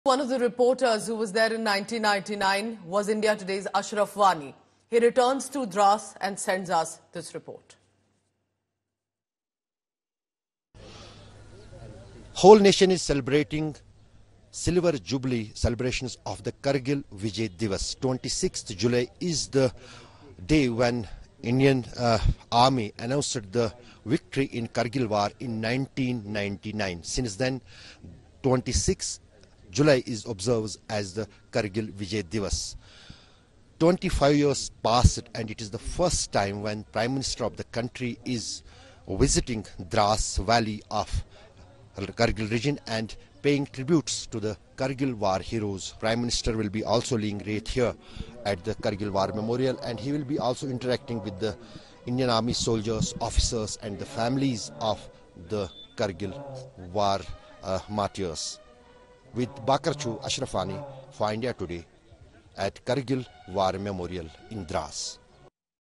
Kargil Vijay Diwas: India Today's Ashraf Wani Tells Heroes' Tales Of Untold Grit
The stage is set for the nation to pay its respects to more than 500 braves who made the supreme sacrifice of their lives ousting the Pakistan Army intruders from these Kargil Heights. Prime Minister Narendra Modi will lead the nation in paying respect to over 500 braves who made the supreme sacrifice of their lives, ousting the Pakistan Army intruders from the Kargil Heights.

So, as India prepares for Kargil Vijay Diwas Silver Jubilee celebrations, India Today's Ashraf Wani brings this ground report from Kargil.